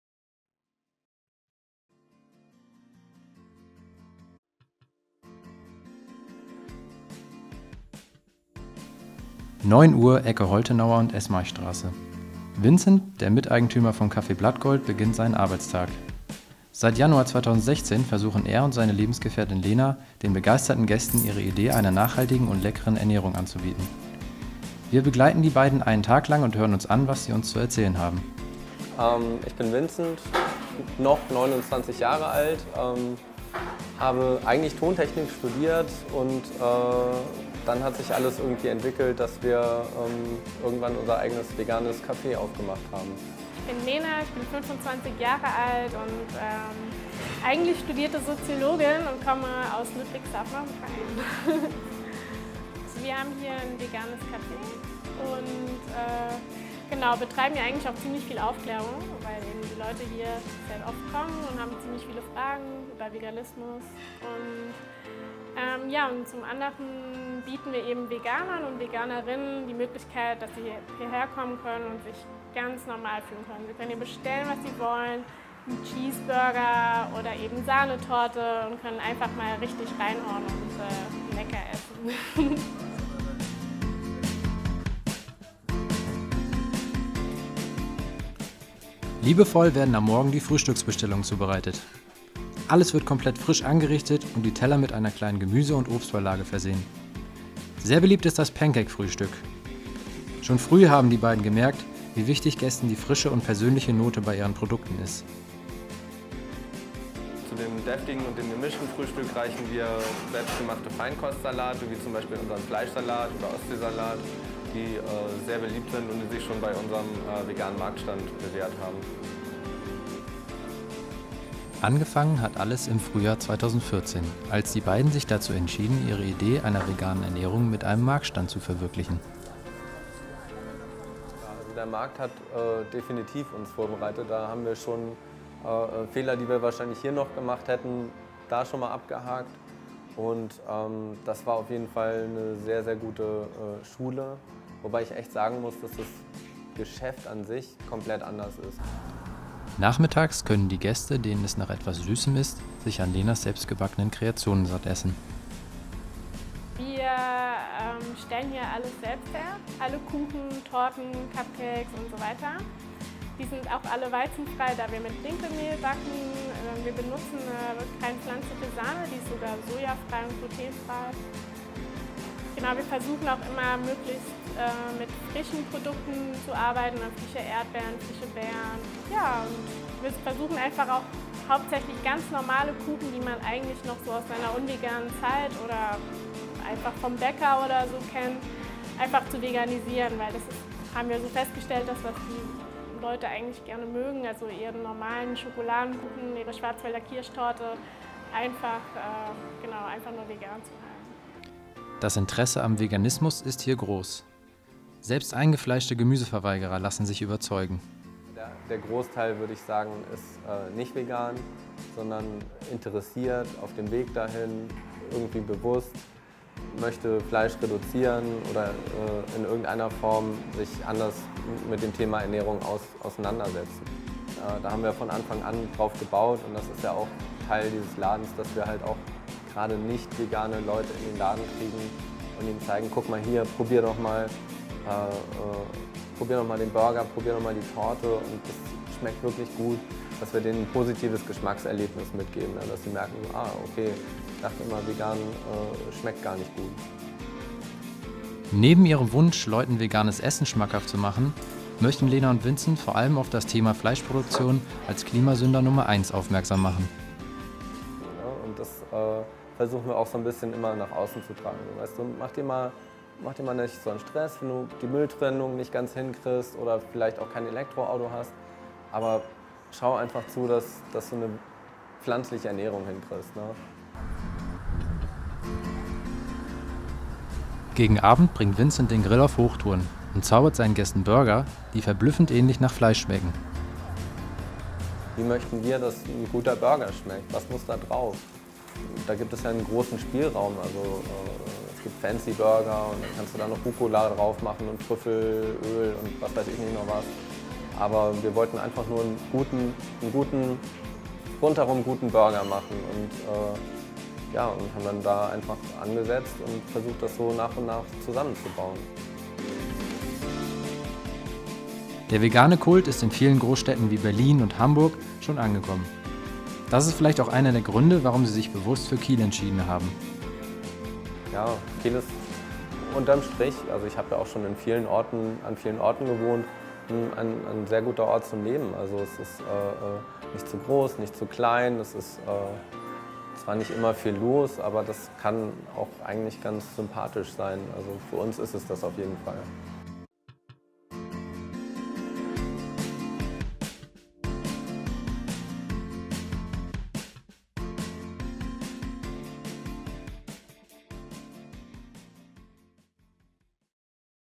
Cafè Blattgold
EINE PRODUKTION DER AG GEOMEDIEN

Ein Film von: Roman Lorbach & Vincent Schröder

Das vegane Café Blattgold wurde am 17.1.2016 in Kiel eröffnet. Die beiden BesitzerInnen Lena und Vincent bieten dort Burger, Kuchen und andere Köstlichkeiten an, die natürlich frisch im Haus zubereitet werden. In diesem Film haben wir die Beiden einen Tag lang begleitet